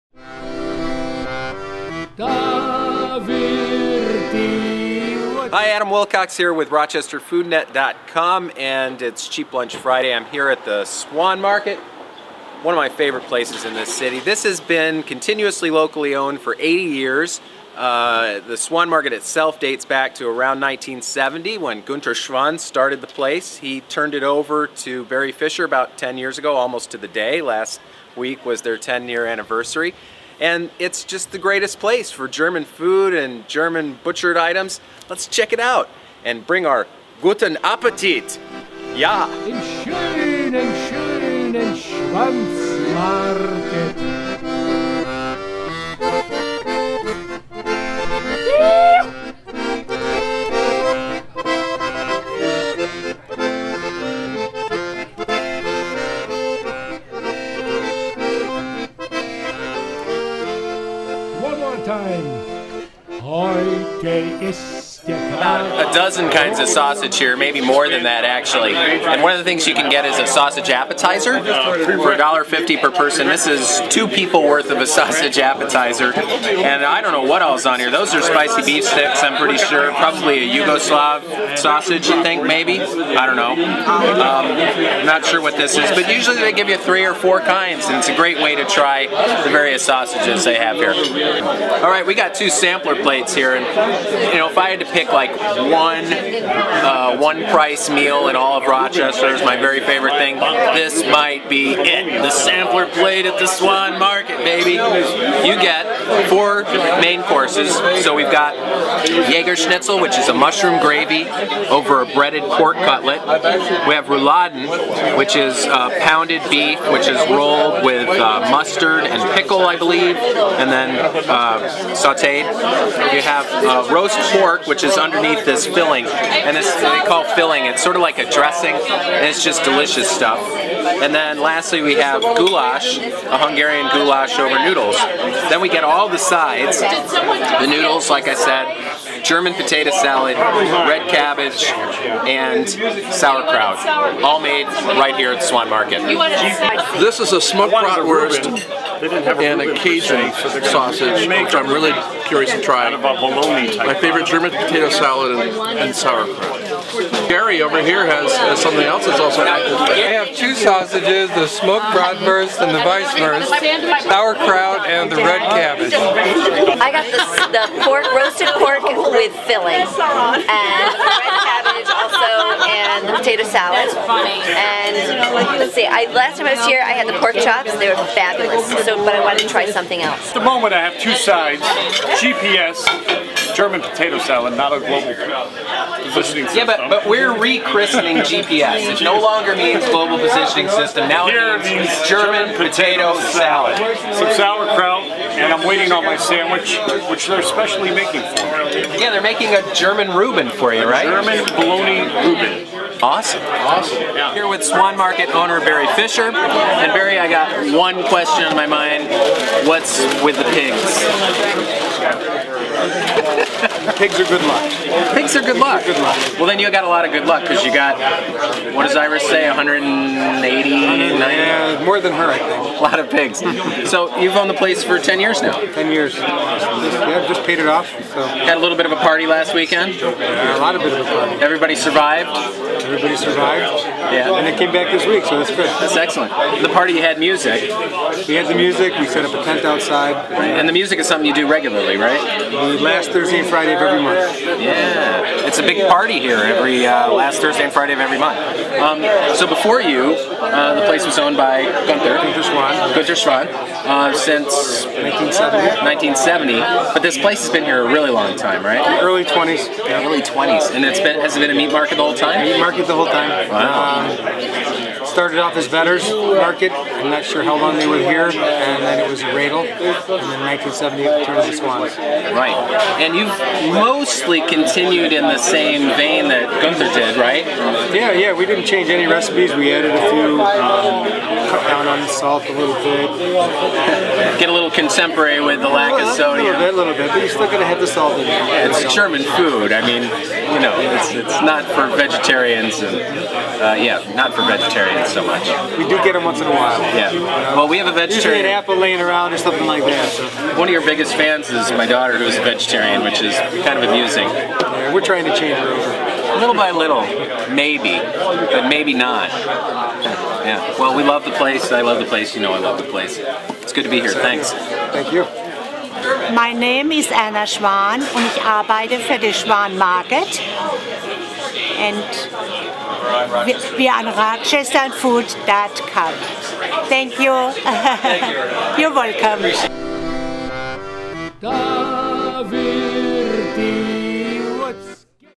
Cheap Lunch: Swan's Market
We love the Swan Market. I wrote about them years ago, and owner Barry Fischer was one of our first ChefTube victims, showing us how to handle a whole pork loin efficiently and  economically. The Cheap Lunch crew headed over this week to check out Swan's amazing German lunches.